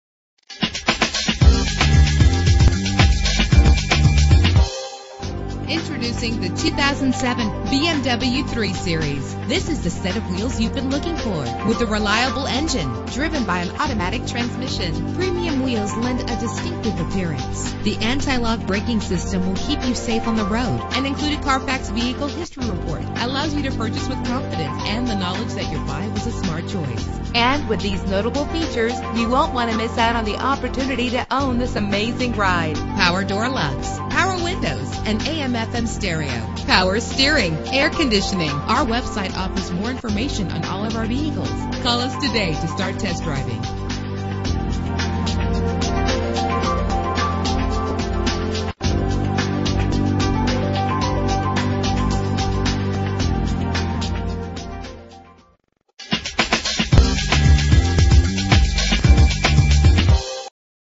2007 BMW 3 Series - EL CAJON CA
Year: 2007
Make: BMW
Model: 3 Series
Trim: 328i Convertible 2D
Engine: 6 cylinder 3.0 Liter
Transmission:
Color: Jet Black
Mileage: 29273
Address:
875 EL CAJON BLVD